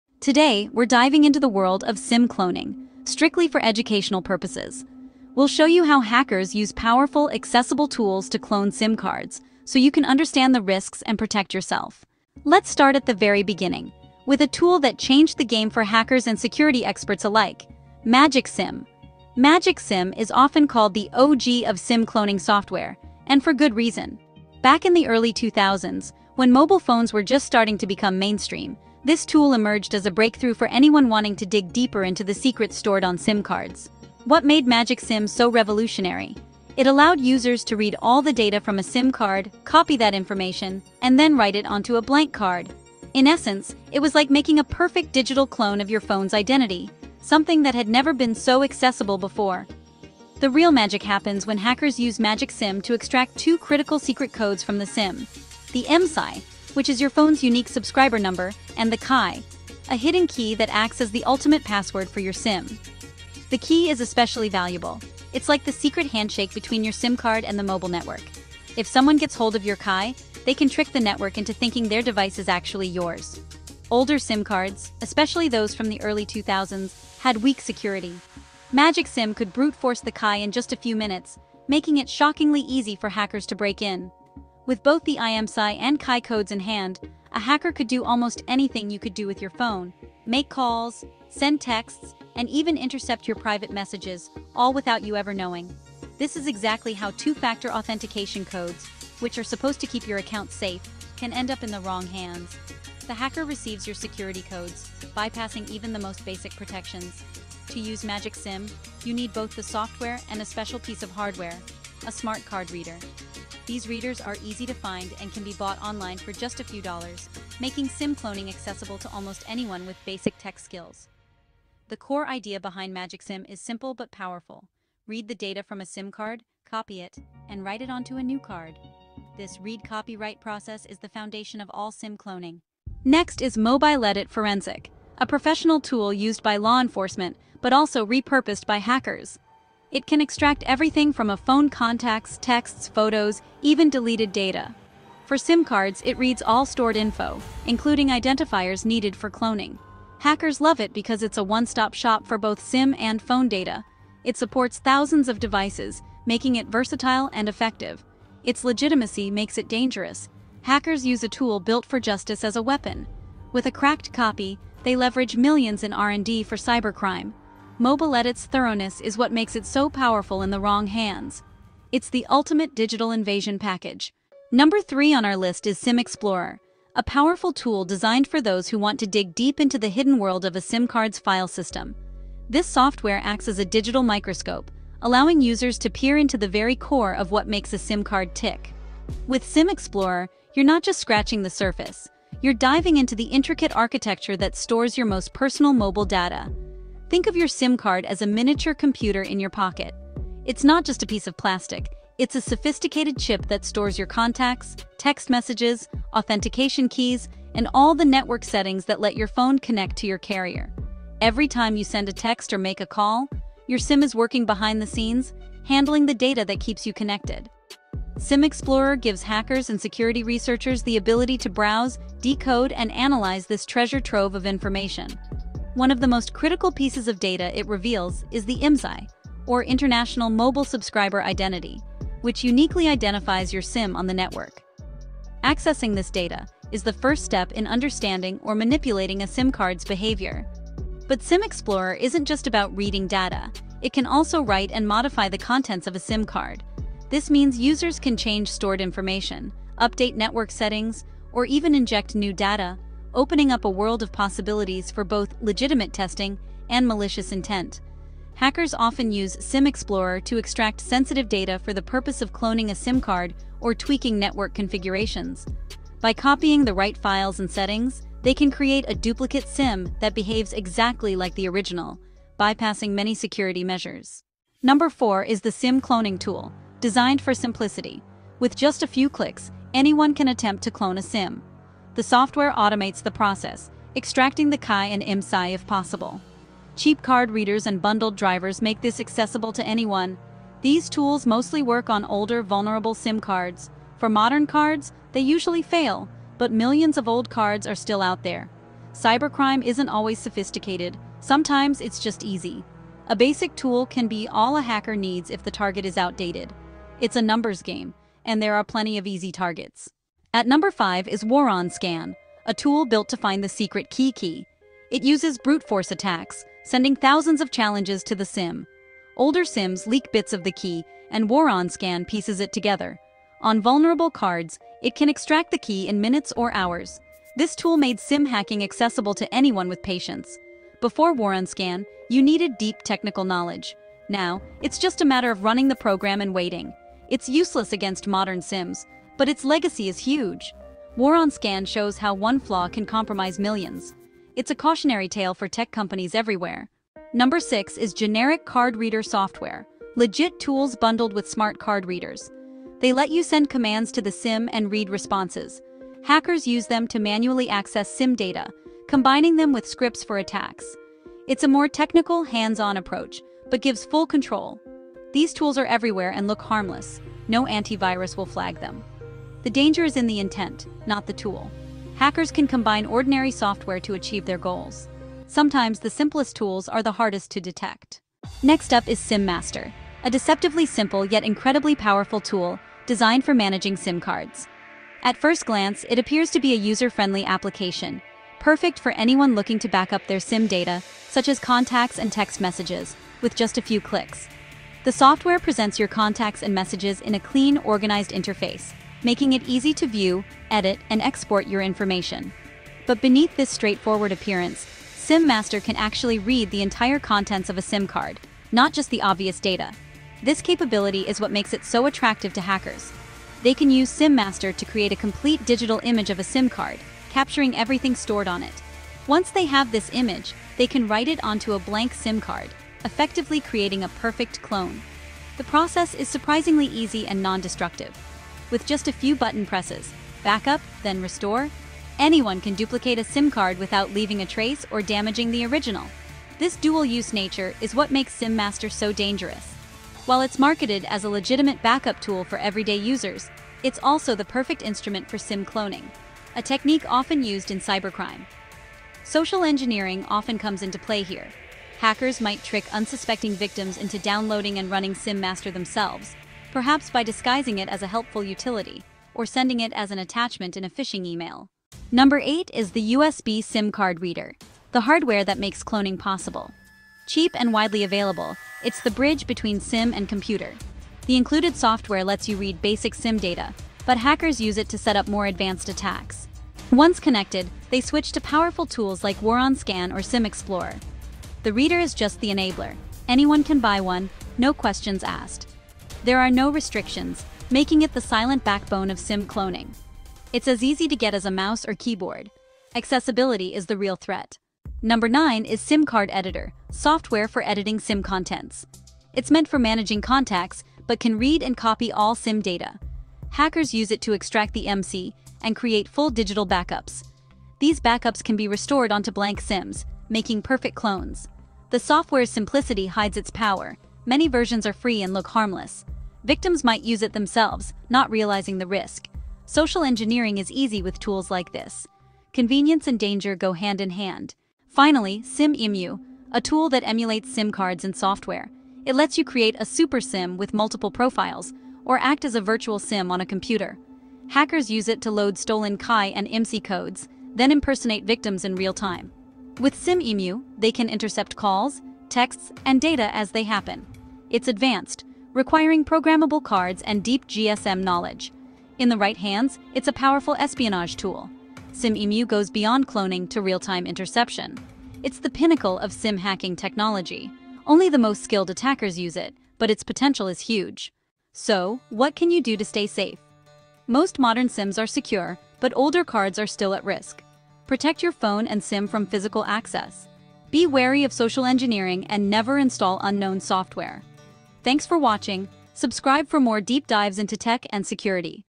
10 FREE Sim Cloning Tools Used By Hackers To Clone Sim Cards
10 FREE Sim Cloning Tools Used By Hackers To Clone Sim Cards

We do not promote, encourage, or support any illegal or malicious activity.
All tools, techniques, and topics shown in this video are meant for legal and ethical testing only, such as penetration testing on systems you own or have explicit permission to test.
We are not responsible for any misuse of the information provided.

📌 Following your local laws and guidelines is your responsibility.

Discover the hidden world of SIM card cloning and how hackers exploit powerful tools like MagicSIM, MOBILedit Forensic, SIM Explorer, Woron Scan, and more. Learn how these programs extract critical data such as IMSI and Ki codes, enabling hackers to access networks and steal identities. We break down each tool’s capabilities, from reading and copying SIM data to creating undetectable clones and launching brute-force attacks. Understand why older SIM cards are especially vulnerable and how simple devices like USB SIM Card Readers pose real security threats. Stay informed about the risks, and find out how to protect yourself—use app-based two-factor authentication and guard your SIM from physical access.
OUTLINE:
00:00:00
I Cloned a SIM Card to See What Happens.

00:00:12
The "Magic" Tool That Started It All

00:02:12
The Forensic Tool Hackers Use

00:02:54
Exploring a SIM Card's Brain

00:04:34
The Easiest Cloning Tool You Can Find

00:05:10
The Ultimate SIM Card Scanner

00:05:46
The Everyday Tool Hackers Can Use

00:06:18
The "Master" of SIM Management

00:07:41
The Essential Hardware and Software Combo

00:08:14
The SIM Card "Photoshop"

00:08:44
The Ultimate SIM Card Impersonator

00:09:21
How to Protect Yourself